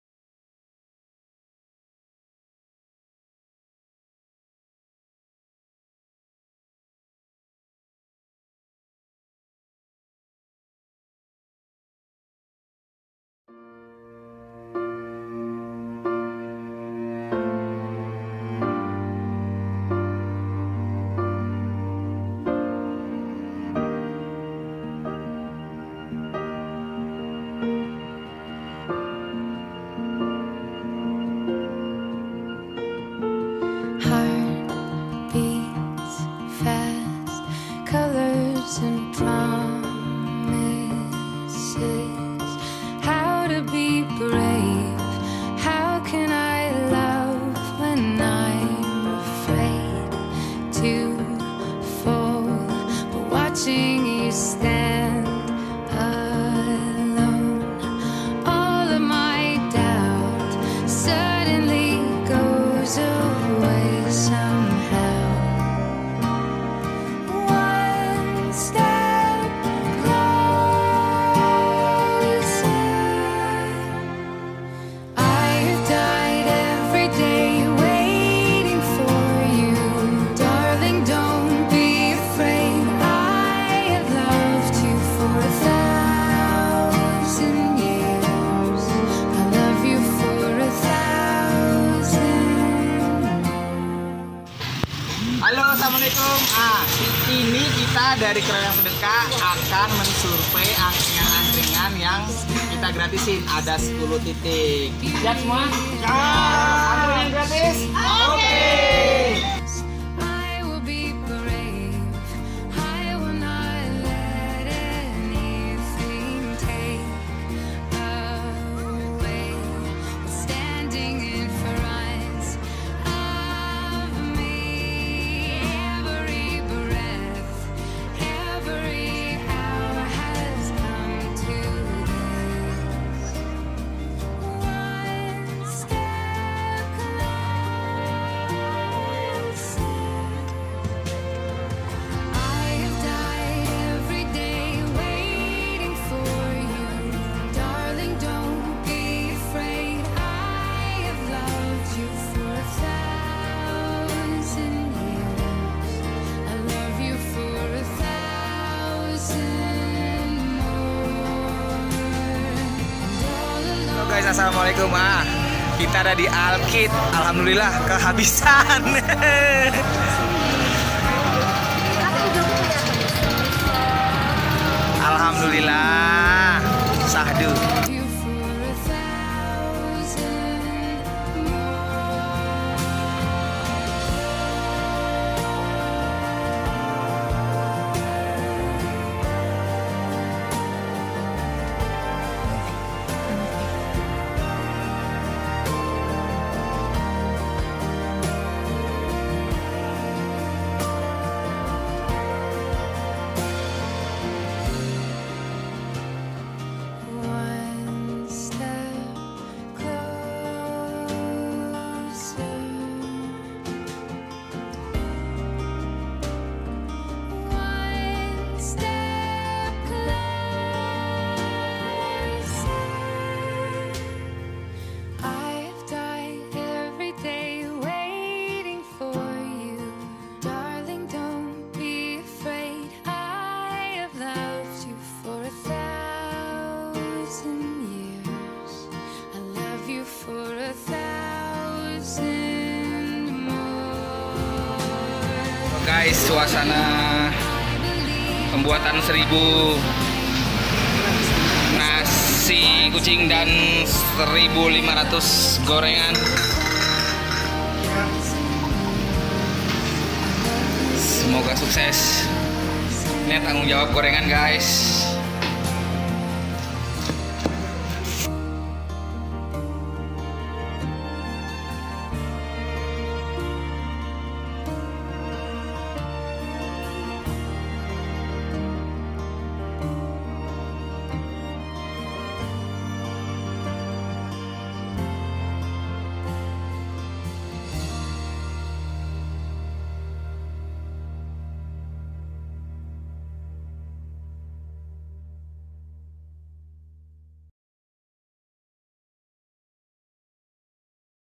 Agkringan Gratis Kroyokan sedekah
Program mengratiskan total 101 angkringan di jogjakarta , tiap minggu 10angkringan.